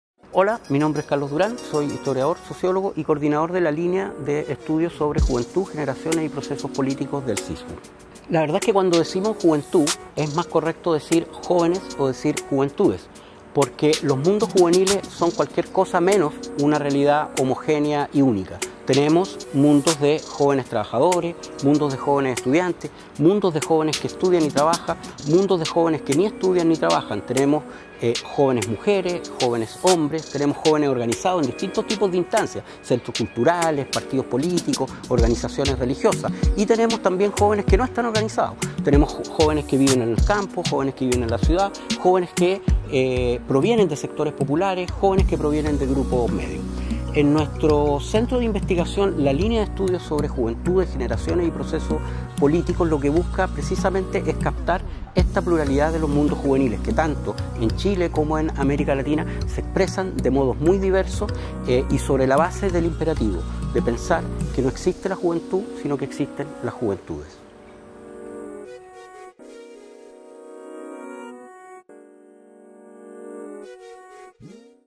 Línea de investigación en Juventud, Generaciones y Procesos Sociopolíticos
Coordinador de la línea de investigación en Juventud, Generaciones y Procesos Sociopolíticos del CISJU, Dr. Carlos Durán, comenta la relevancia de los estudios en juventud como campo de estudio y área de desarrollo de nuestras y nuestros investigadores.